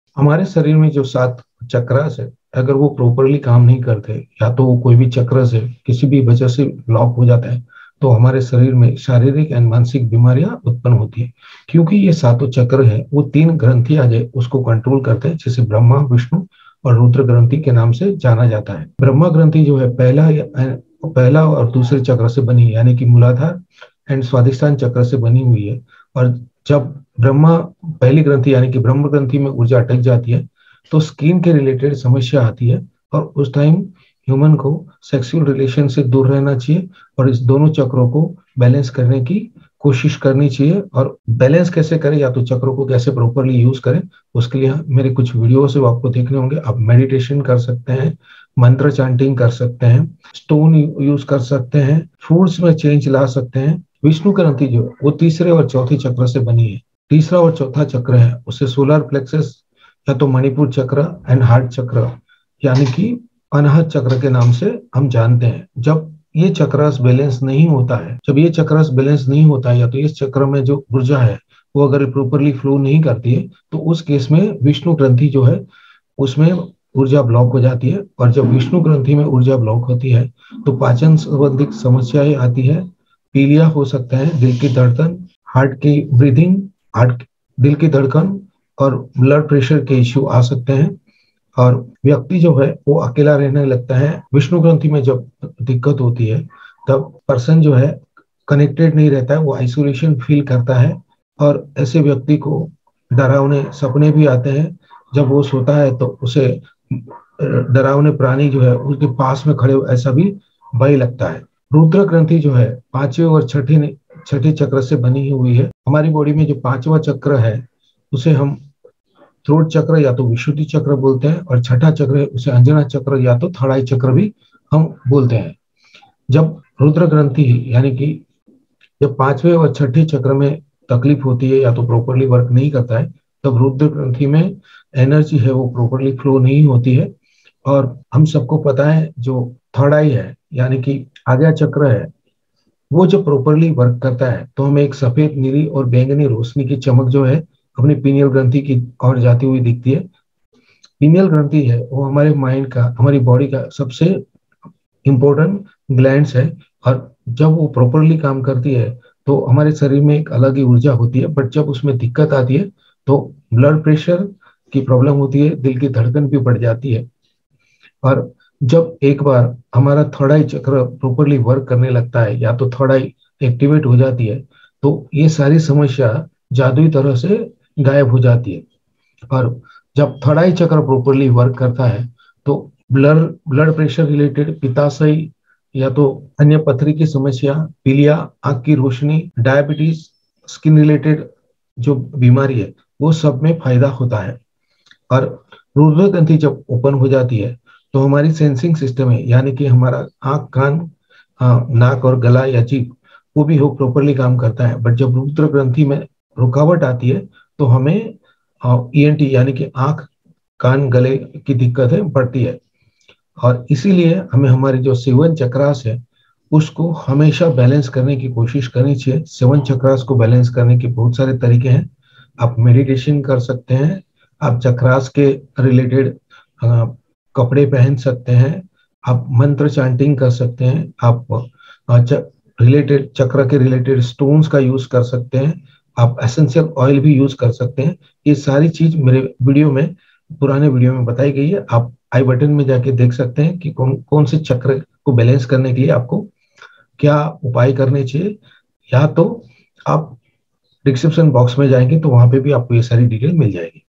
सात चक्र ब्रह्मा विष्णु रुद्र ग्रंथि saat chakra brahma vishnu rudra granthi
चक्र के प्रत्येक जागरण के साथ लक्षण प्रकट नहीं होते हैं, लेकिन ब्रह्मा, विष्णु और रुद्र नामक तीन ग्रन्थियों (मानसिक गांठों) के खुलने के साथ।
तीन ग्रन्थियों में प्राण की रुकावट के कारण दिखाई देने वाले लक्षण:-

ब्रह्म ग्रंथी पहले और दूसरे चक्रों से बनी है यदि पहले ग्रंथी - ब्रह्म में ऊर्जा अटकी हुई है, तो जांघों के भीतरी क्षेत्र में त्वचा की समस्या होगी या सामान्य त्वचा की समस्याएं होंगी। इस उदगम के दौरान हस्तमैथुन और सेक्स से बचना चाहिए।
ब्रह्म ग्रंथी के खुलने से त्वचा संबंधी समस्याएं कम हो जाएंगी।

विष्णु ग्रंथी तीसरे और चौथे चक्र से बनी है, अगर विष्णु ग्रंथी में ऊर्जा अटकी हुई है, तो पाचन संबंधी अत्यधिक समस्याएं होंगी और इससे पीलिया हो सकता है। दिल की धड़कन और उच्च रक्तचाप में वृद्धि होगी। यदि विष्णु ग्रंथी में विशेष रूप से हृदय चक्र में रुकावट है तो व्यक्ति एक ज़ोंबी की तरह आगे बढ़ेगा। इस रुकावट को 'आत्मा की अंधेरी रात' के रूप में भी जाना जाता है। कोई दुःस्वप्न को वास्तविक और अकेले सोने से डरता हुआ देख सकता है। वह सांप और जंगली जानवरों जैसे शेर, बाघ या भेड़िये को अपने बगल में खड़ा देख सकता है।

रुद्र ग्रंथी 5वें और 6वें (तीसरे नेत्र) चक्रों से बनी है, गले में खराश की समस्या होगी। एक बार जब ऊर्जा तीसरी आंख (छठे चक्र) को छूती है तो सफेद, नीली और बैंगनी रोशनी की चमक होगी क्योंकि इससे पीनियल ग्रंथि खुल जाती है जो मस्तिष्क का एक भौतिक अंग है। इससे उच्च रक्तचाप और दिल की धड़कन भी बढ़ जाएगी

एक बार तीसरा नेत्र खुल जाने पर सभी समस्याएं जादुई रूप से दूर हो जाएंगी।
रुद्र ग्रंथी खुलने के बाद जिन समस्याओं का समाधान होगा उनकी सूची इस प्रकार है:-
उच्च रक्तचाप, पित्ताशय की थैली या गुर्दे की पथरी, पीलिया, आंखों की रोशनी, मधुमेह, त्वचा की समस्याएं किसी भी तरह के न्यूरोसिस या अवसाद।
क्यों?
रुद्र ग्रंथी के खुलने से संवेदी तंत्र बिना मध्यस्थ यानी चेतन मन के सीधे संवेदी आउटलेट्स (आंख, नाक, कान और जीभ) के साथ काम करता है।